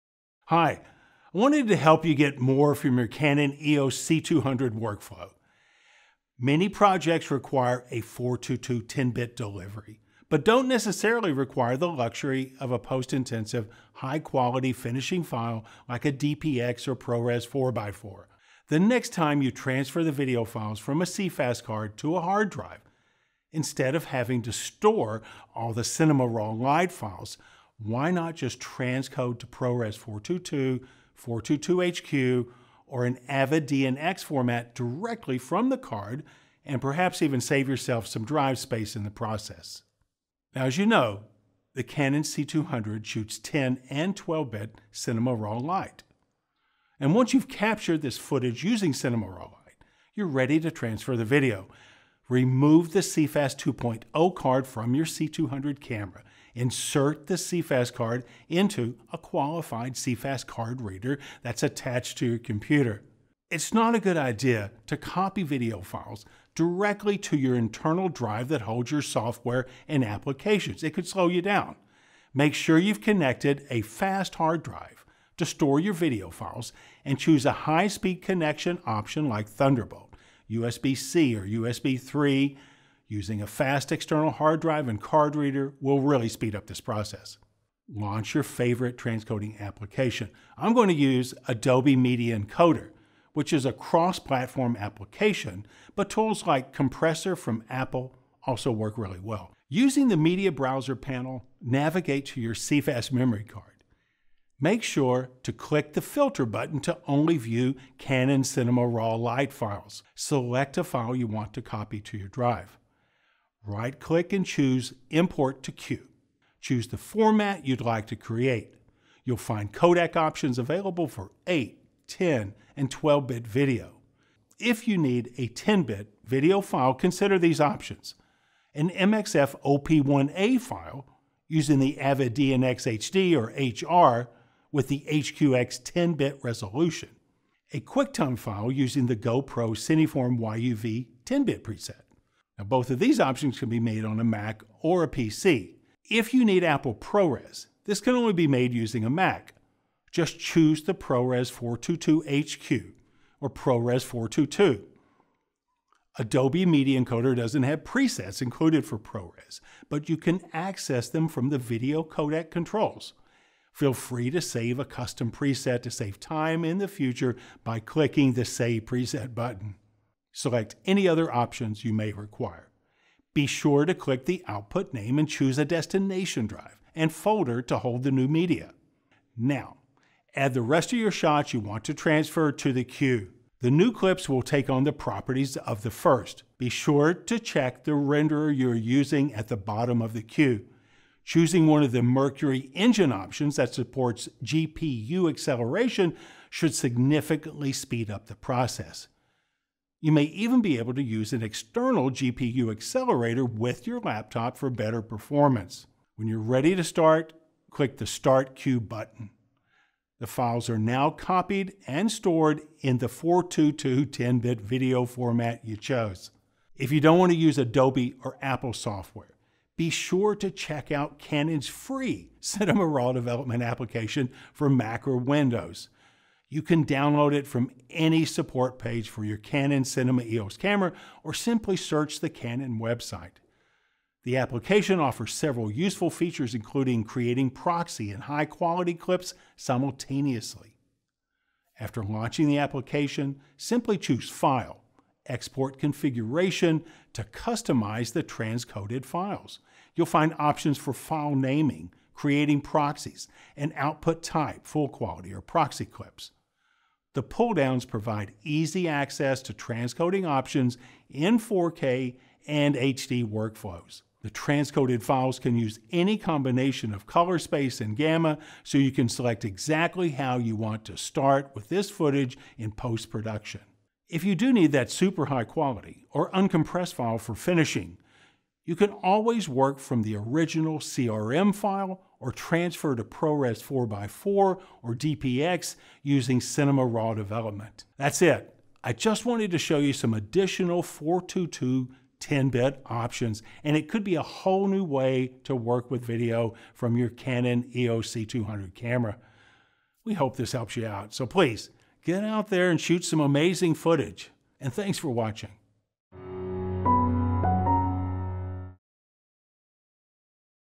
How-To: Transcoding Options for the Canon EOS C200
Not all projects require a RAW workflow. In this video we look at popular options for transcoding Canon RAW Light to a variety of 4:2:2 10-bit file type alternatives.